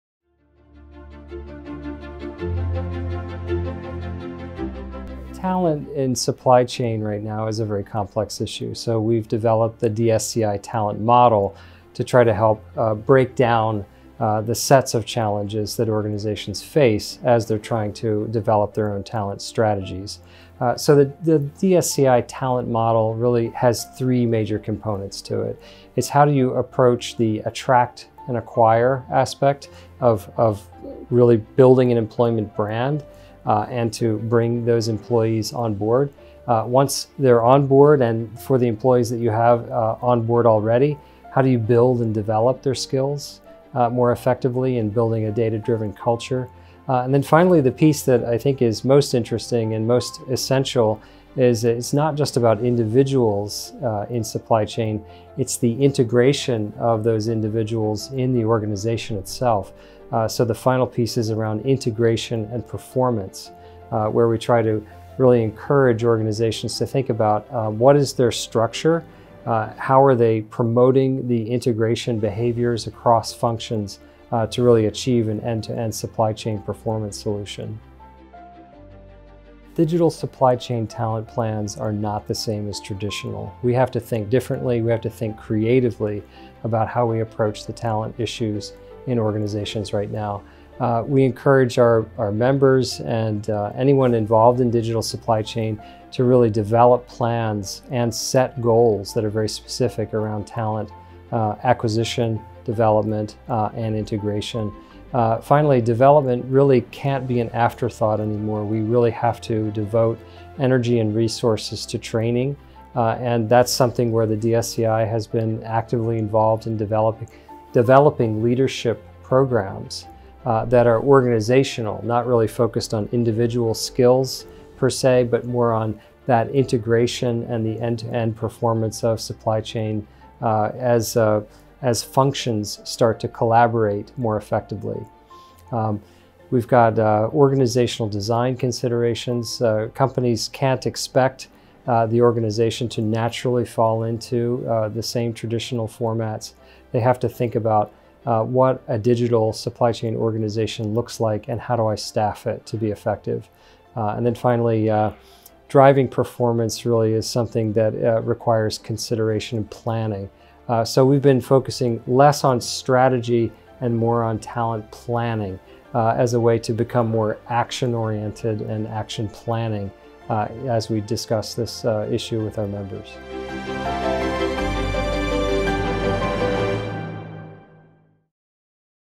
DSCI Talent Model: Acquisition, Development And Integration
Dave Kurz, Research Fellow, DSCI discusses DSCI's New Talent Model, sets of challenges that organizations face and how to build an "Employment Brand". Dave made these remarks during the Digital Supply Chain Institute's 7th Executive Leadership Forum in Belgrade, Serbia. Around 70 supply chain executives attended the DSCI's seventh forum in September 2019.